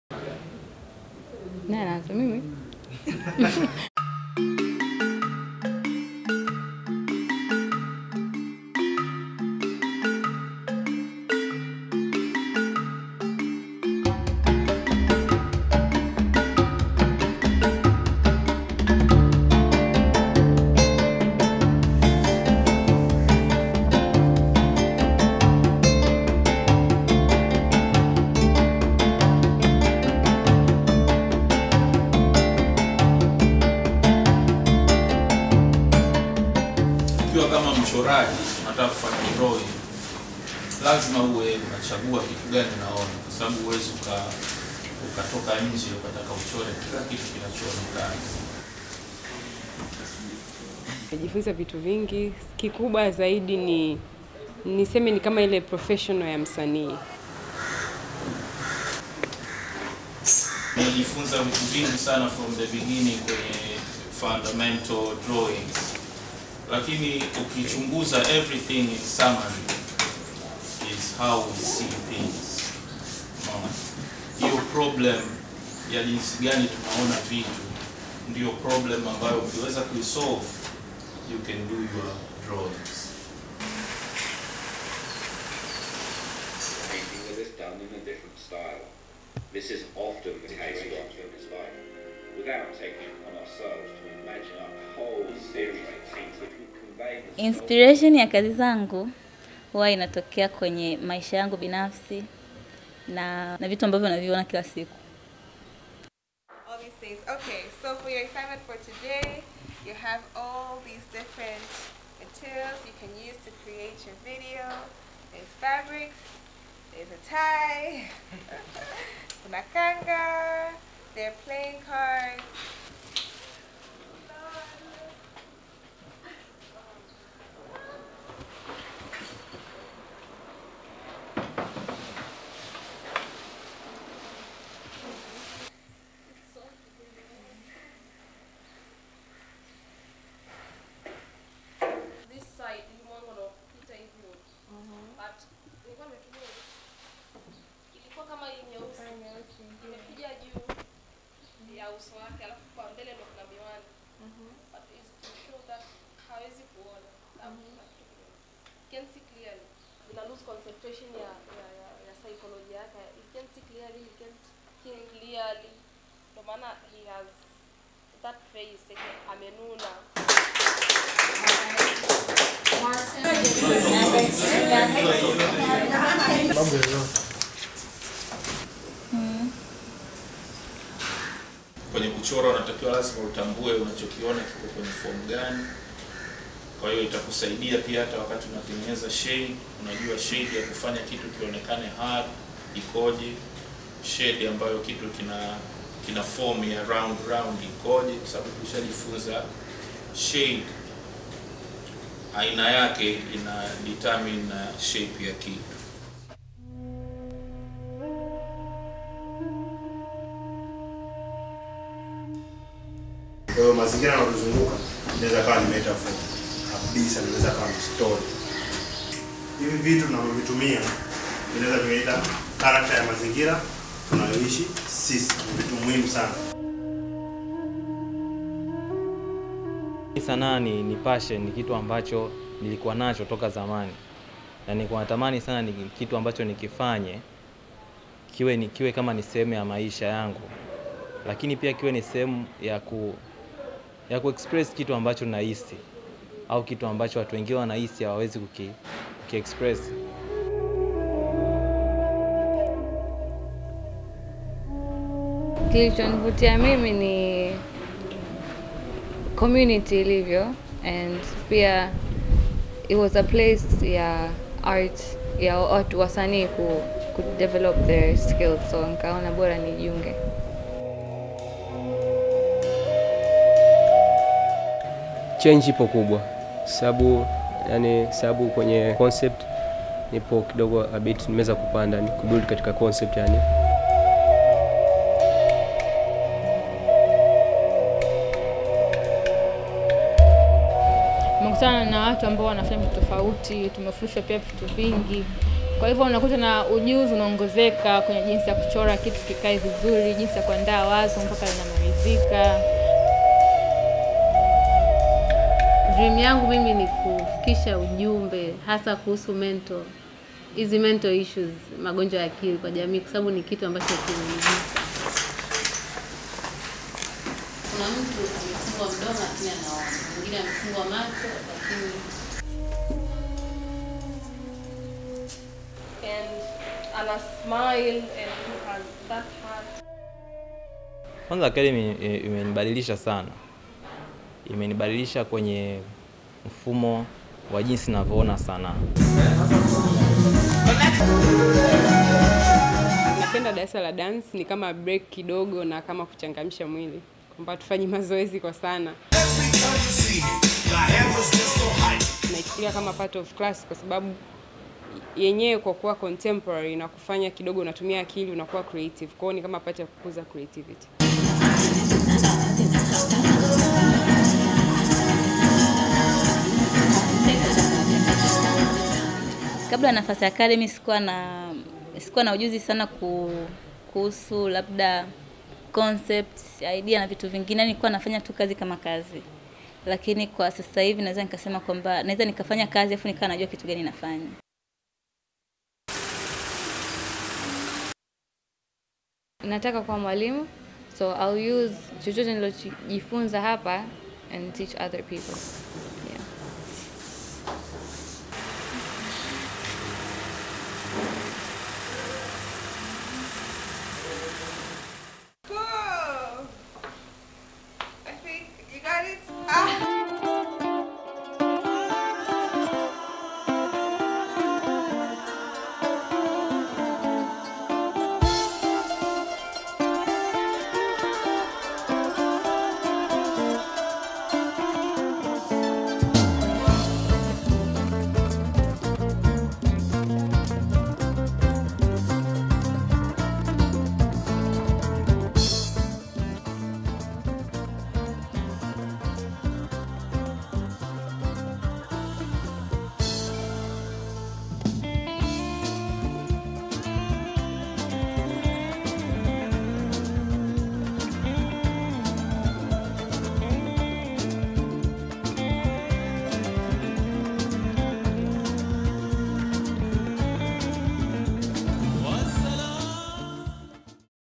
The Nafasi Academy: An Education (2020)
In 2020, the Nafasi Academy for Contemporary Art and Expression was born in Dar es Salaam, Tanzania. Developed within the context of Nafasi Art Space, the Academy was a year-long programme of inquiry-driven arts education that encouraged artistic growth, experimentation, conceptual engagement, and collaboration.

Our first year of programming supported a cohort of visual artists as they embarked on their journeys towards professionalizing their contemporary practices. The curriculum and design of the Academy was developed over several years by the Nafasi team with consistent community collaboration and input. The course included 5 intensive weeklong masterclasses focused on theoretical and thematic areas including identity, history, process, audience, and entrepreneurship, each followed by 4-6 weeks of mentorship emphasising technical skills in drawing, painting, printmaking, photography, digital art, video art, and physical expression.

This video, directed by Nuru Media, focuses on the mentorship aspect of the programme, and features Academy Mentors Paul Ndunguru, Gadi Ramadan, Valerie Amani, and Tadhi Alawi, as well as the Nafasi Academy artists.